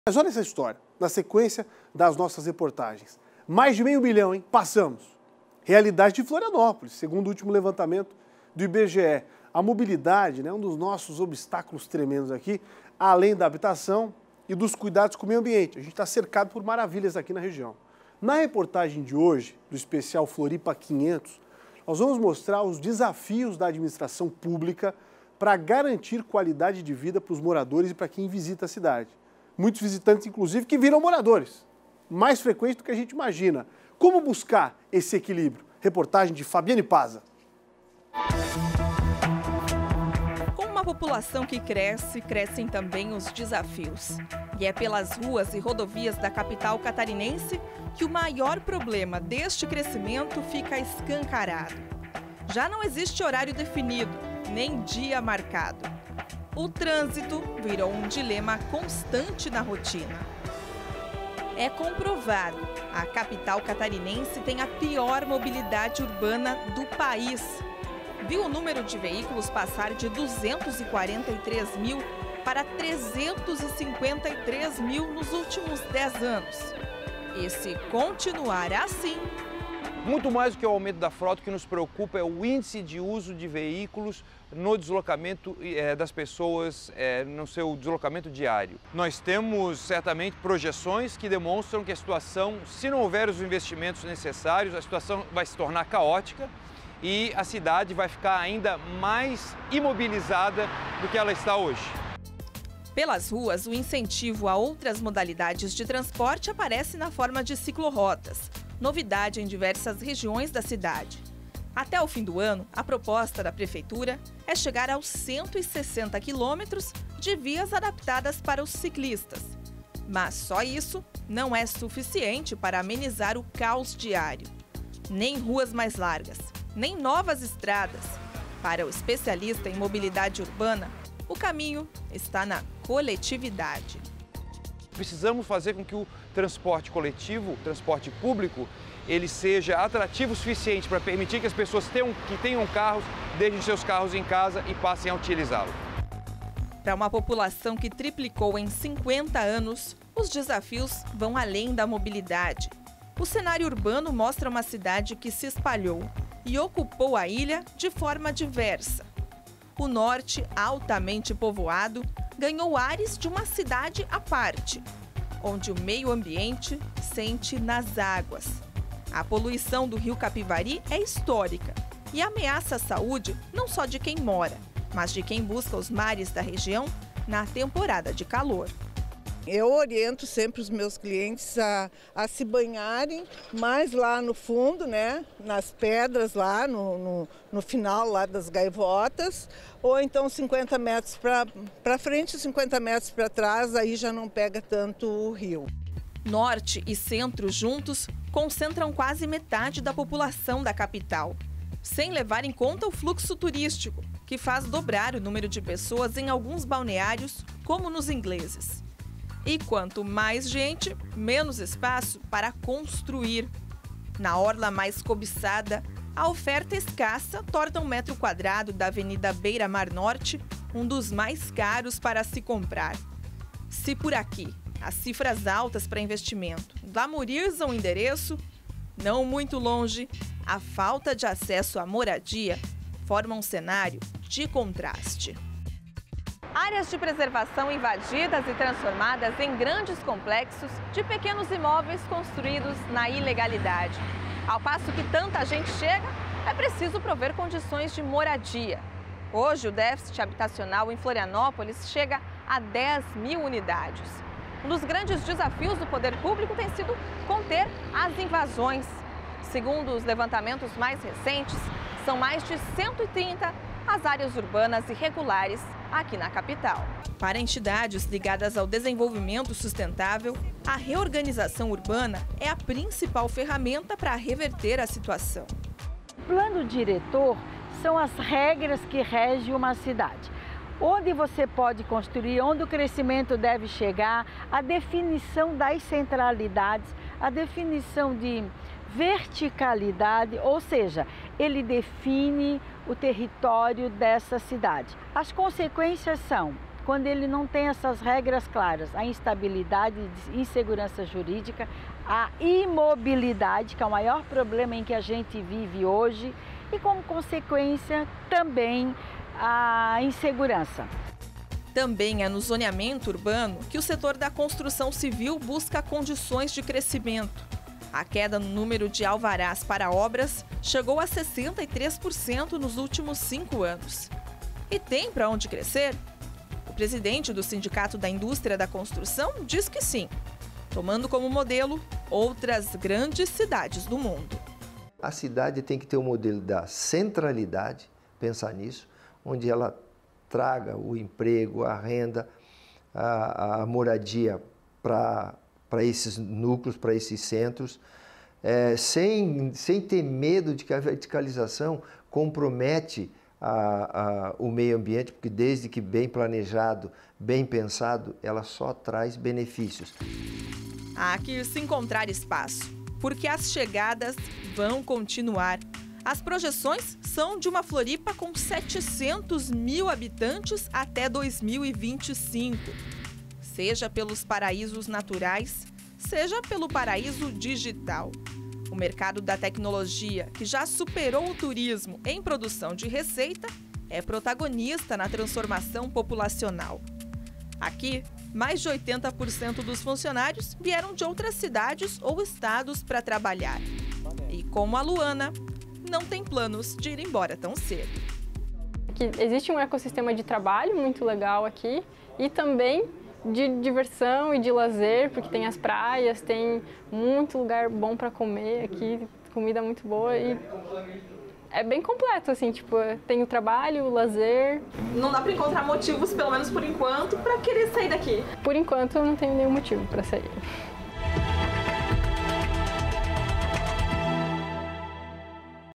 Os desafios para atender mais de 500 mil habitantes de Florianópolis
O último levantamento do IBGE pontoo mais de meio milhão de habitantes em Florianópolis. Um dos principais problemas da cidade é a mobilidade, além de habitação e descaso com o meio ambiente.  Na reportagem dessa terça-feira (22), nossa equipe da RICTV | Record vai mostrar os desafios da administração pública para garantir qualidade de vida aos moradores e visitantes.